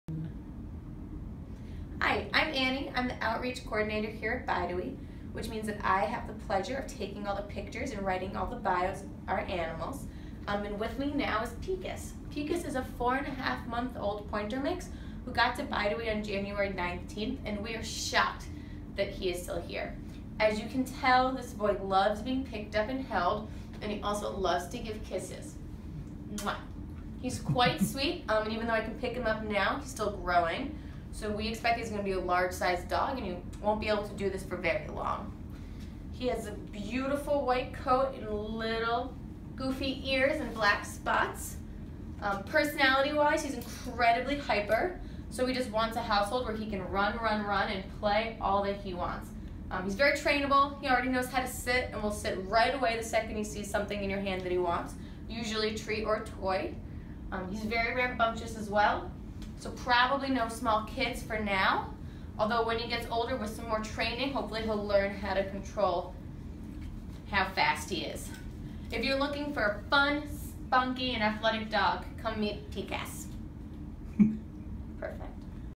The Staff Pick of the Litter for April is Pecas!
“Pecas is April’s Pick of the Litter.” Pecas is a four and a half month old Pointer mix who came to Bideawee in February. Pecas is my Pick of the Litter because this puppy is just so fun! Pecas is full of pizazz. He’s energetic, spunky, clever, and sweet. He loves to play and currently his favorite activity is to just run as fast as possible. He’ll gladly chase toys, although he prefers a game of tag over a game of fetch. He’s incredibly smart and highly trainable. He already knows how to sit for a treat and if he sees anything good in your hands, he’ll plop his butt right down and give you those big pleading puppy dog eyes. Pecas is also very sweet, when you pick him up into your arms, his energy level calms and he becomes a kissing machine. He loves to be around people and will definitely be a fabulous cuddle buddy. Pecas’ dream home has an active owner who is as excited for playtime as he is. He’s a bit rambunctious right now so we recommend no young children in the house. Pecas adores other dogs and is very extroverted with them. Because he’s so playful, we recommend a meet and greet if you have a dog at home. If you’re looking for a dog that will provide endless entertainment and unconditional love, then come meet Pecas!”